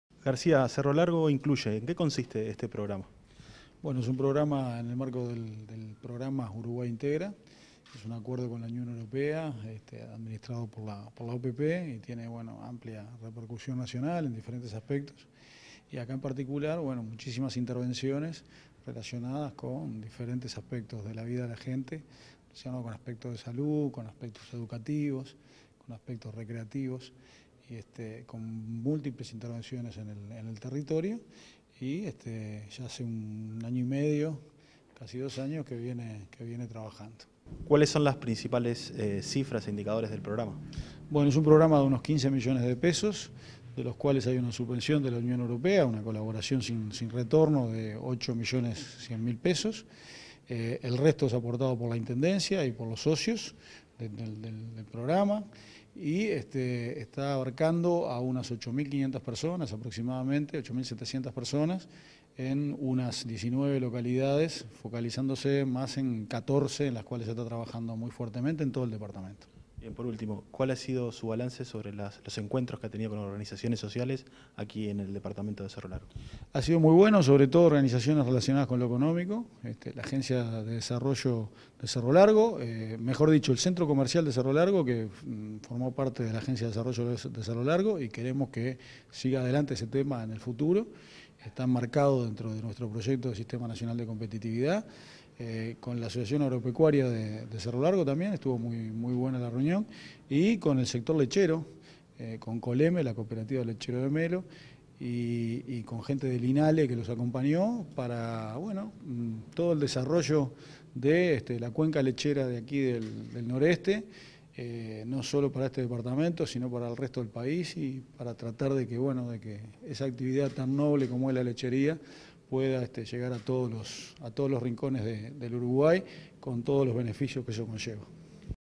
OPP facilita realización de estudios médicos y capacitación laboral de población rural
El director de OPP, Álvaro García, se reunió en la ciudad de Melo con el equipo técnico del programa Cerro Largo Incluye. García resaltó que la iniciativa facilita el acceso de la población rural a los servicios de salud, y a capacitación laboral además de apoyar eventos en que se privilegia la identidad local. El programa cuenta con un presupuesto de $ 15 millones, de los cuales más de la mitad son gestionados por OPP.